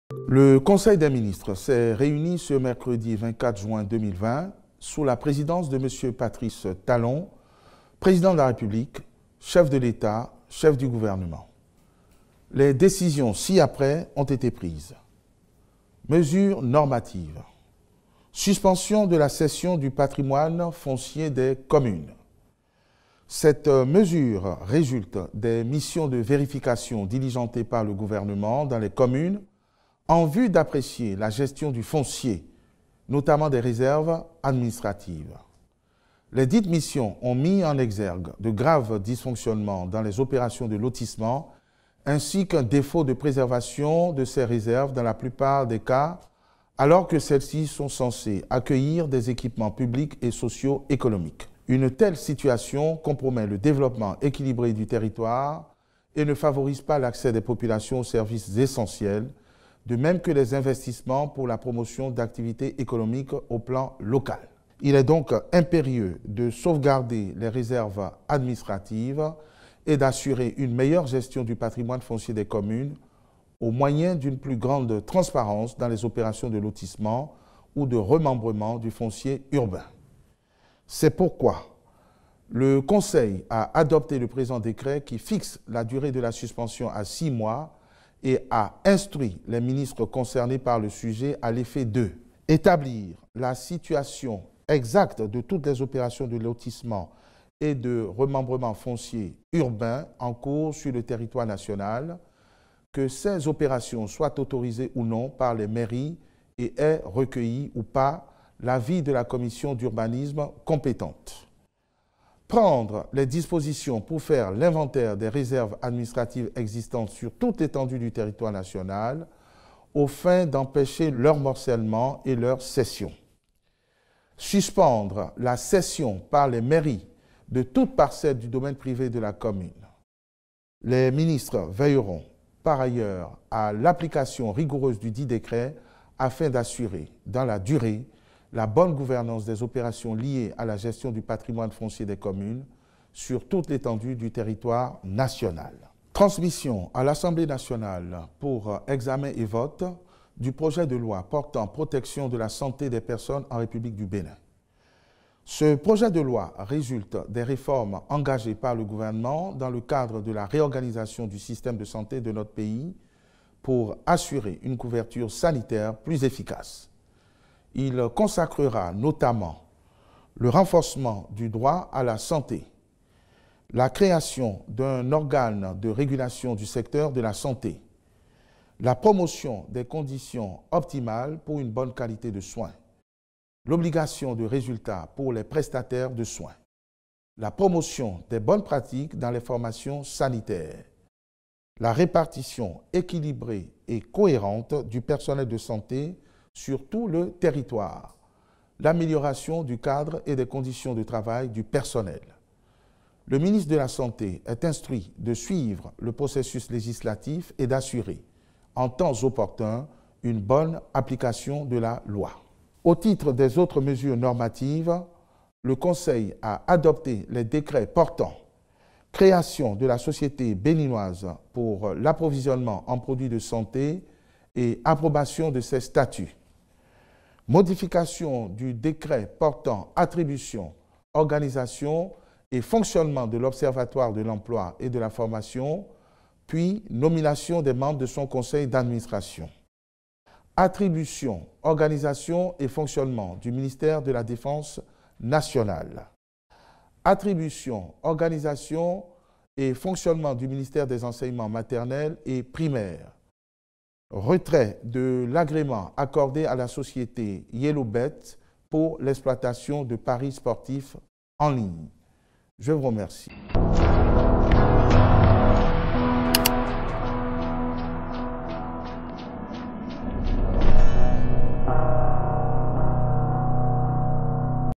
Bénin : compte-rendu du conseil des ministres du mercredi 24 juin 2020
Le Conseil des ministres s’est réuni ce mercredi 24 juin 2020, sous la présidence effective du Chef de l’Etat, Patrice Talon. Compte-rendu du ministre de la communication et de la poste, Alain Orounla.